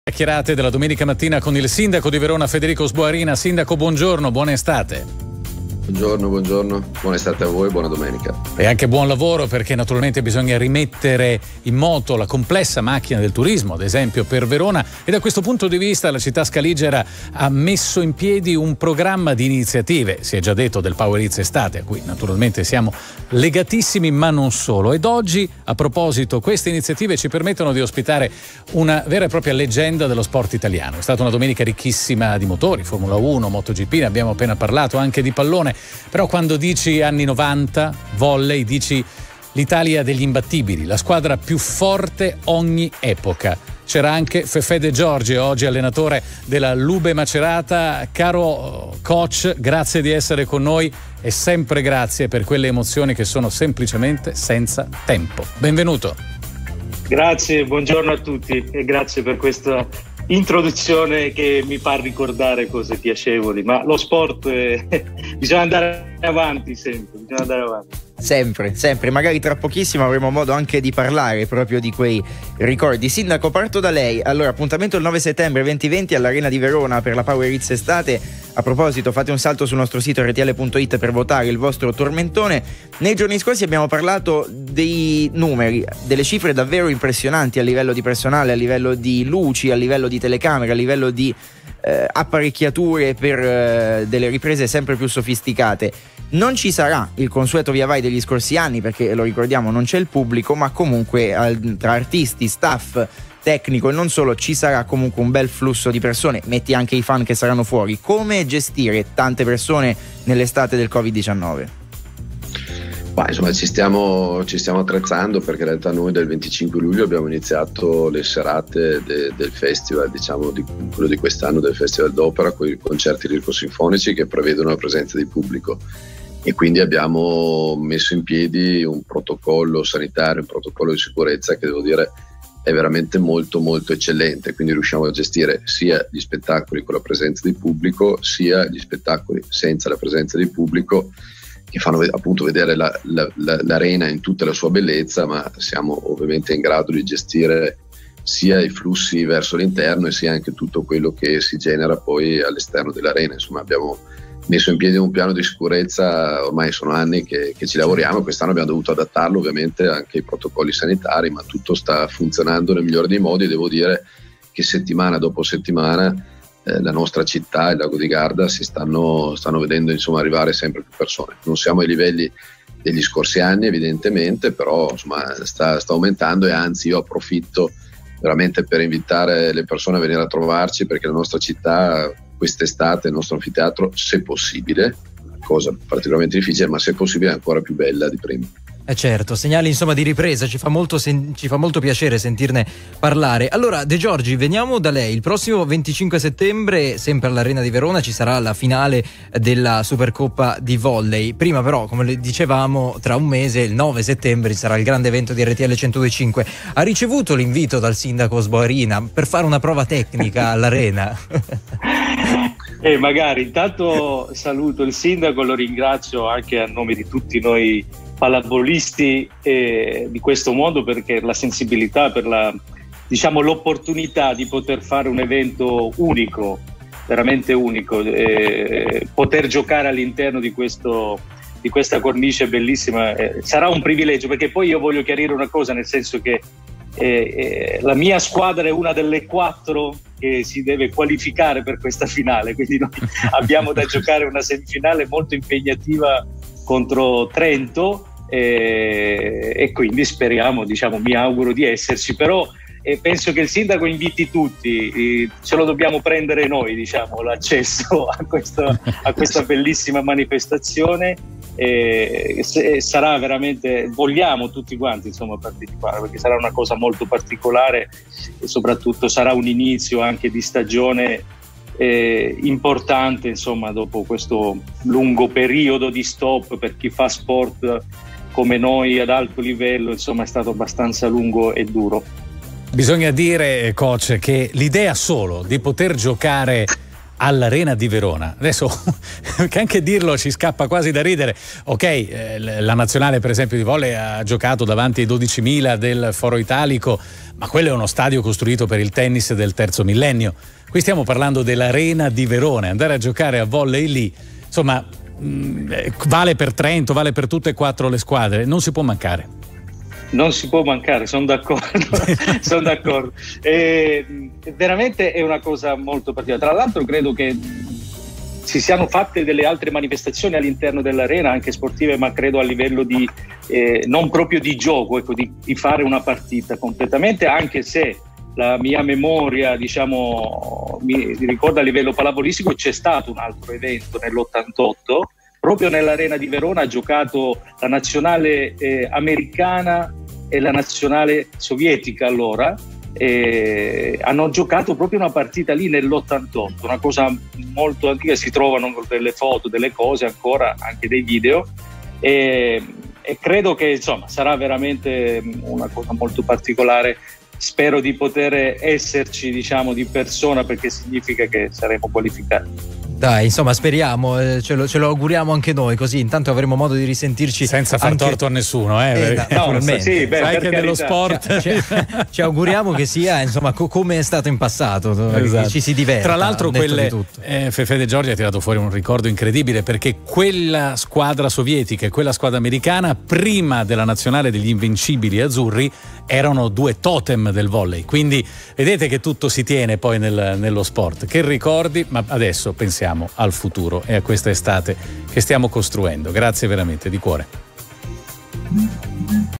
09-08-2020: Del Monte Supercoppa a Verona su RTL 102.5
Del Monte Supercoppa a Verona su RTL 102.5 col Sindaco di Verona Sboarina e Fefè De Giorgi, tecnico della Cucine Lube Civitanova.